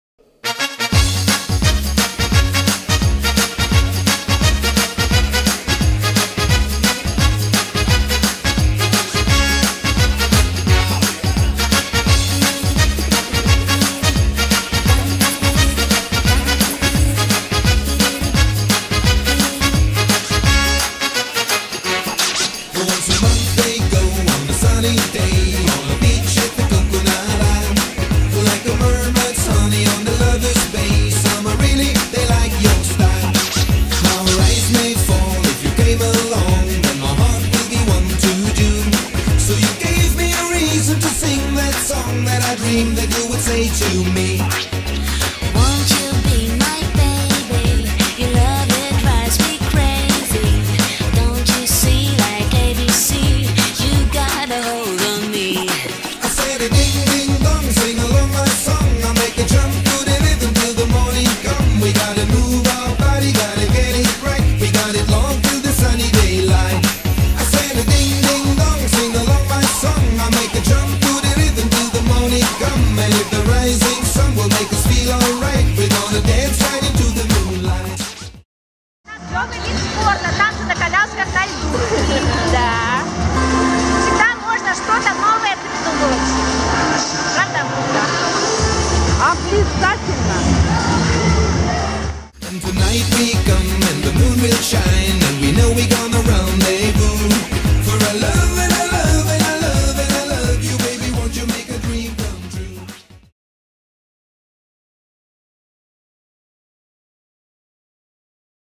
Wheelchair dancing on ice
On the wheelchair can dance not only the floor but also on ice!
На инвалидной коляске можно танцевать не только на паркете, но и на льду!